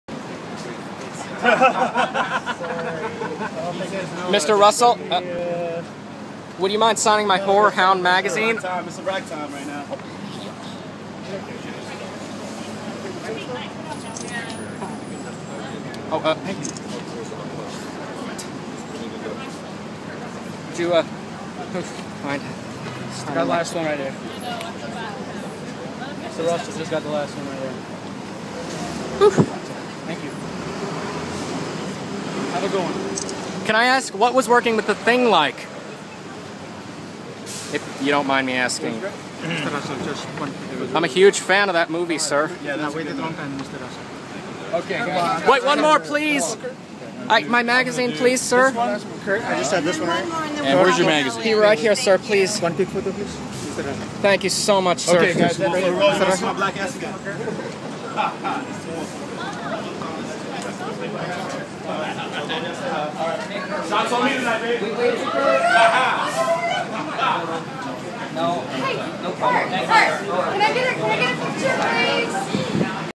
Kurt Russell Signing Autographs At His Hotel
I was able to get him to sin my Horror Hound Magazine!